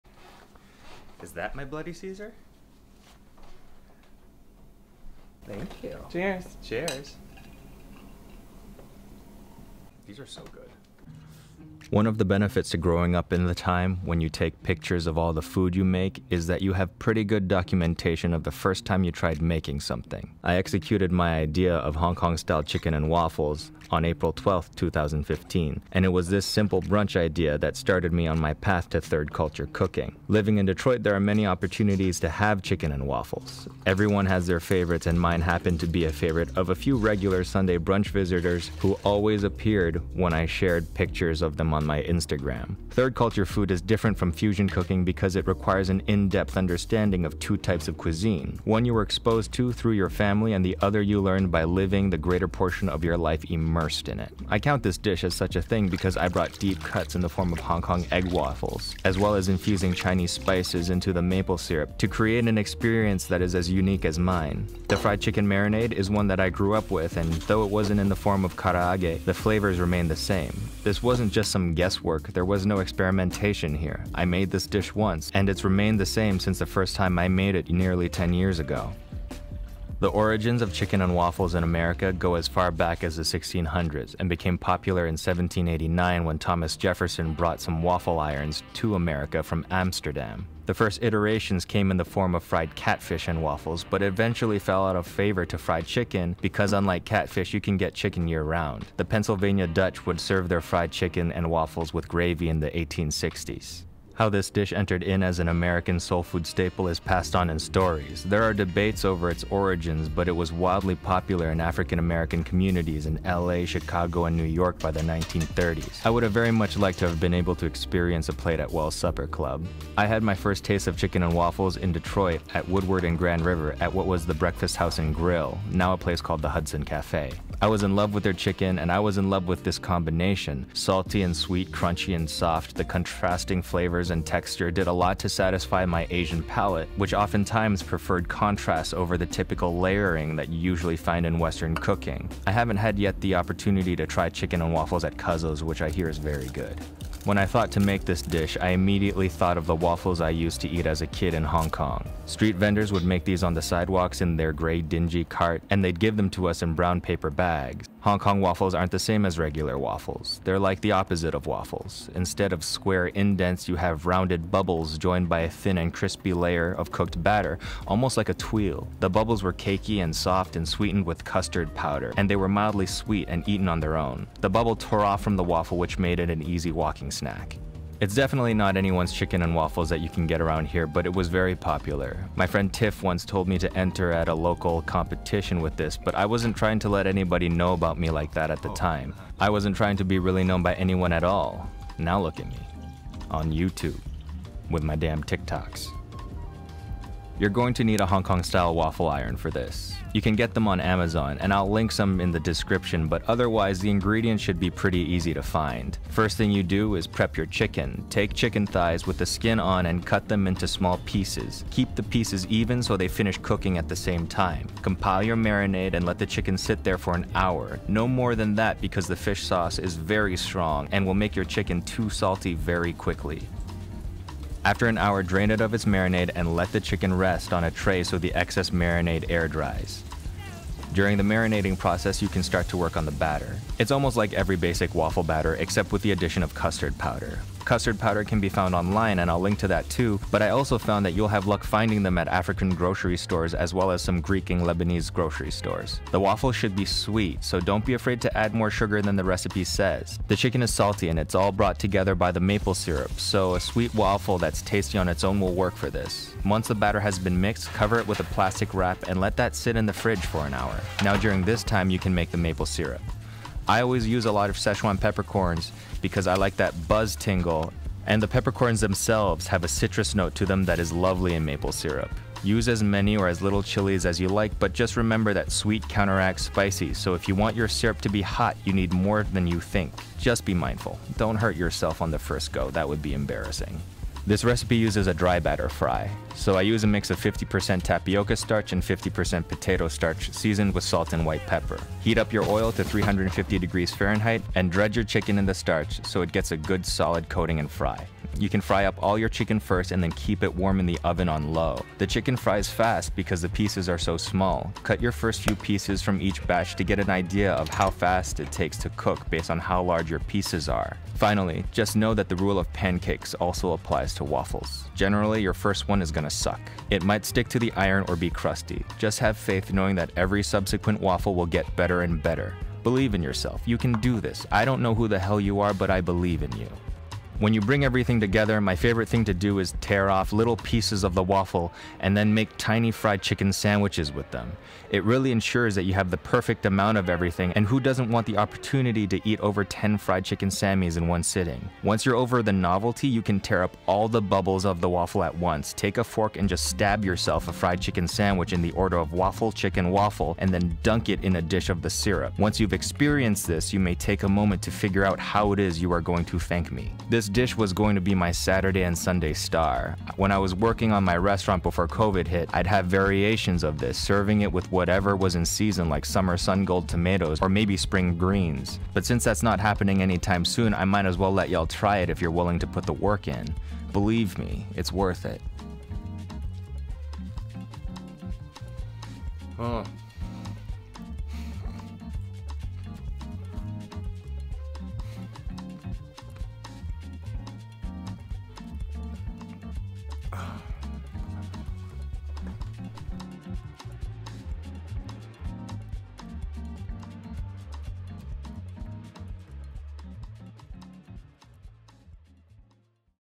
House Chicken and Waffles with Szechuan Peppercorn and Chili Maple Syrup
Items Required:
Hong Kong Style Waffle/Eggette Maker

Custard Powder

Recipe:
Waffle Batter

* 1 cup all-purpose flour
* 1 tsp  baking powder
* 1/2 tbsp tapioca starch
* 1  tbsp custard powder
* 2 large eggs
* 2/3  cup  granulated white sugar
* 1 tbsp milk powder (optional)
* 150 ml  milk
* 1 tbsp vegetable oil
* 1 tsp vanilla extract

Directions

Note: Save time by preparing the batter and chicken close to the same time. They both need an hour of rest time so doing them as close as you can together will save you some downtime.

To get the timing of this I would have everything ready and while the chicken is frying in the oil I’ll put the ready batter in the waffle maker and both should be ready around the same time. But let your chicken drain on a paper towel for a minute first.

* Whisk  all the ingredients until only very small lumps remain. Set batter in the fridge for one hour.
* Set the waffle maker to 350º and allow to preheat
* If your iron is non stick there’s no need for oil otherwise brush with oil or butter
* Pour a cup of batter into the iron, close the iron and flip
* Set a timer for 3 minutes
* Once the timer is done check the iron, if the waffle pulls out early its done otherwise add continue cooking for 30 second increments
* The bubbles should be soft but fire and the tulle should be pliable until it has had a chance to dry till crispy
For the karaage

Ingredients
* 5 Chicken thighs - skin on
* 1/2 cup Tapioca flour
* 1/2 cup Potato Starch
* 1 Tbsp Salt
* 1 tbsp White Pepper

Marinade
* 2 tbsp Fish sauce
* 3 tbsp Sugar
* 1/3 cup Soy sauce
* 1/3 cup Xiaosing wine

Directions

* Mix all the marinate ingredients together and set aside
* Cut the chicken thighs into small and even bite sized pieces
* Place the pieces into the marinate and allow to sit for one hour
* After they’ve marinated remove the chicken and place on a paper tower and allow to air dry for 30 minutes
* Combine your starches and flowers along with salt and white pepper and mix well
* Preheat your frying oil to 350ºF
* Press the chicken into the dry batter to get it nice and stuck on the chicken pieces
* Fry the pieces until golden, they’re small so they won’t take as long as bone in fried chicken
* Allow the excess oil to absorb into a paper towel

For the Maple Syrup

1 Bottle of maple syrup
1/2 cup of Szechuan or Korean chili flakes
3 tbsp of Szechuan peppercorns

Directions

* Heat the maple syrup on medium till simmering and add the peppercorns and allow to sit for 5 minutes
* Add the chili flakes and turn it up till it bubbles, watch it closely as it might overflow the pan
* Once it starts tubule up remove from the heat and let it sit for 10 minutes
* Strain into a heat safe container, glass bottle if you fancy
* Store in the Fridge